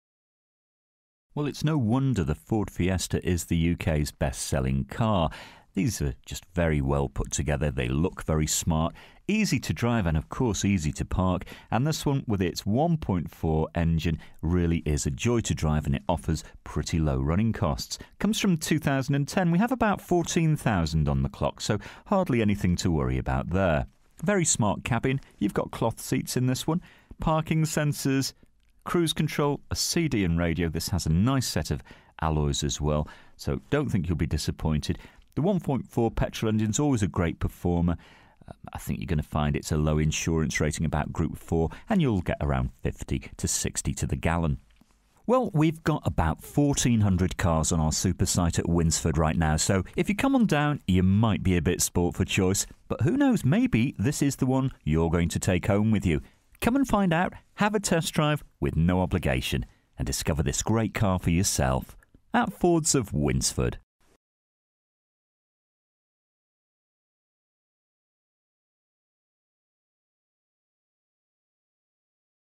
FORD FIESTA HATCHBACK (2010) 1.4 TITANIUM 5DR - SK10WBP
DETAILS:
First registered: 09/03/2010
Current mileage: 14430
Registration: SK10WBP
Colour: GREY

FEATURES:
front electric windows, remote central locking, cloth interior, all round airbags, climate control, pas, abs, alloy wheels, metallic paint, cd, parking control, parcel shelf present, cruise contro

This used car is for sale at Fords of Winsford Car Supermarket in Cheshire, UK